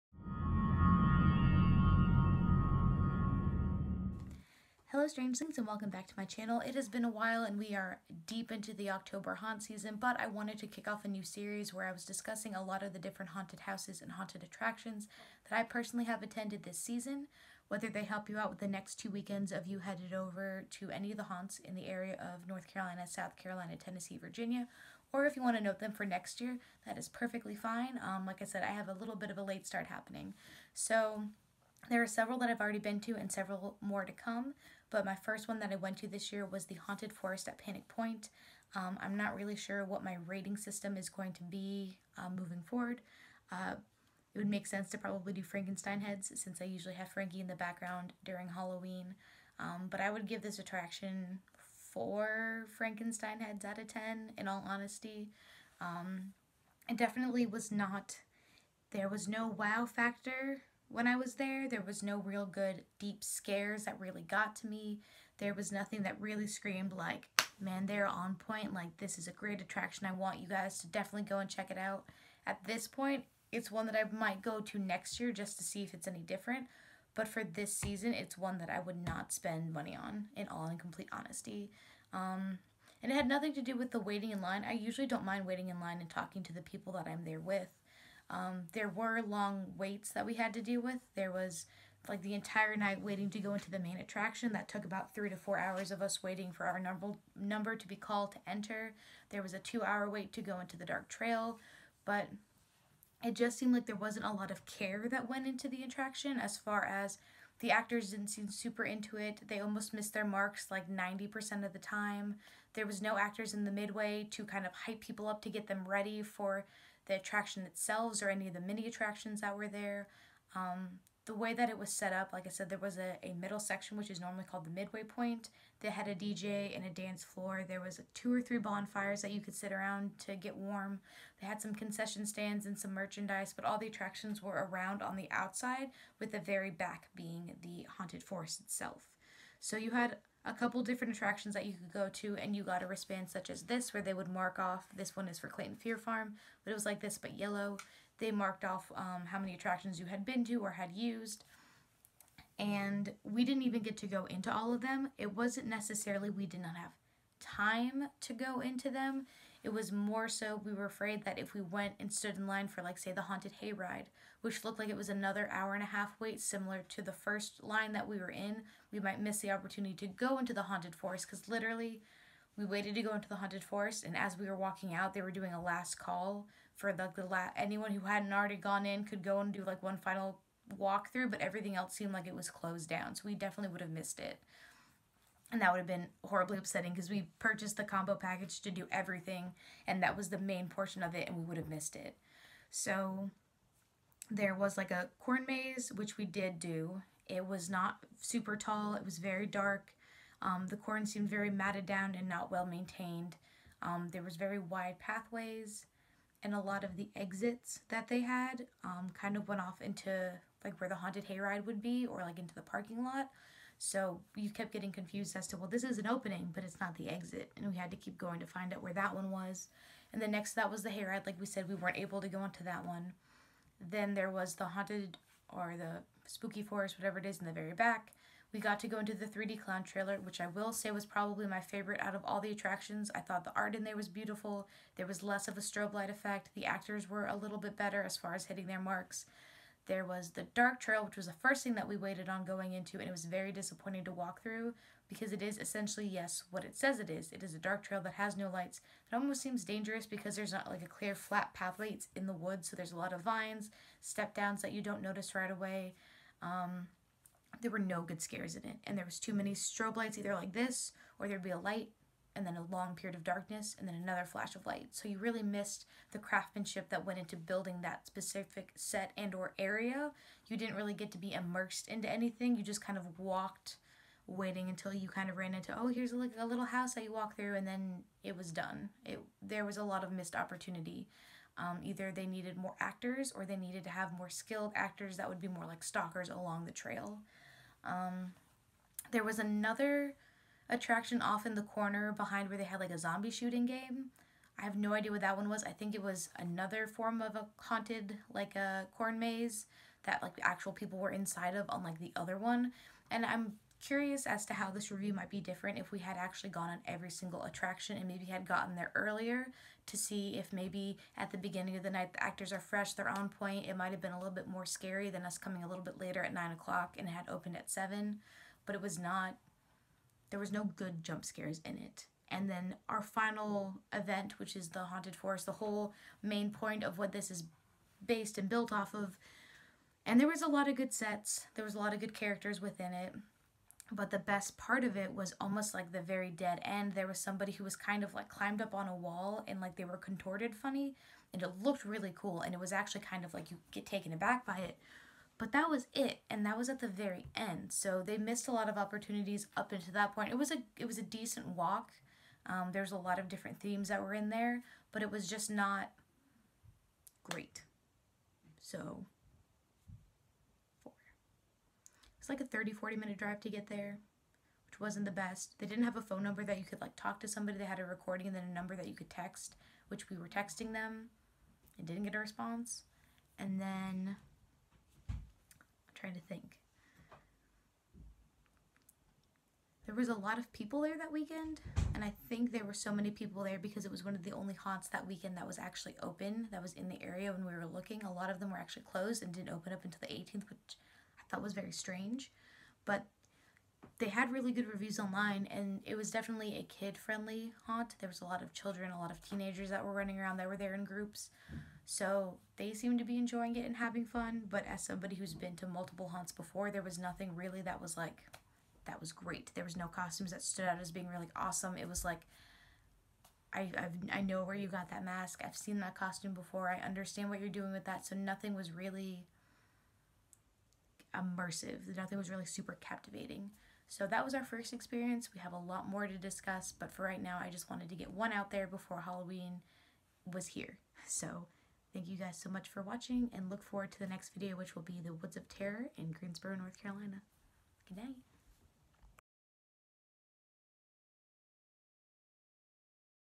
Haunt Review: Haunted Forest at Panic Point!
Hello Stranglings! May I introduce my newest video series to you - reviews on local Haunts on my area. Whether they are some you have looked into attending or want to see what other haunts are out there then this is the video for you! Let me know about your favorite local haunt or if you have attended one that I review!
Haunted Forest at Panic Point
Storenvy: EmilyNicoleStrange.Storenvy.com